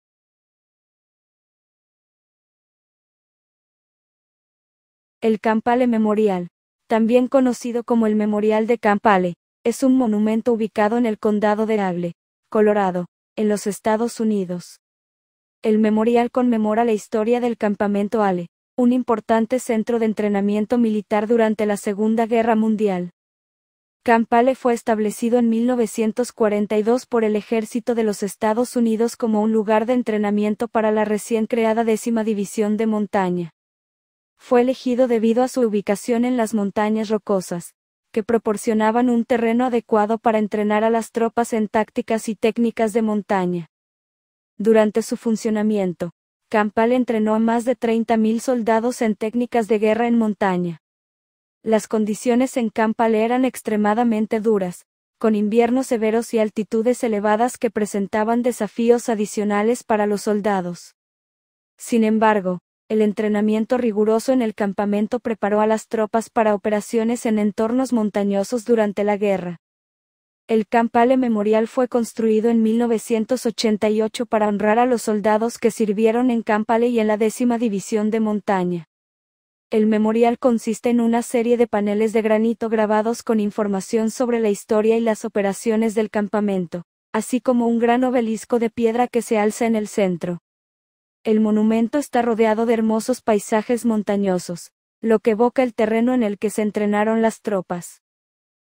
Camp Hale Memorial En Leadville, CO
El Camp Hale Memorial, también conocido como el Memorial de Camp Hale, es un monumento ubicado en el condado de Eagle, Colorado, en los Estados Unidos. El memorial conmemora la historia del Campamento Hale, un importante centro de entrenamiento militar durante la Segunda Guerra Mundial.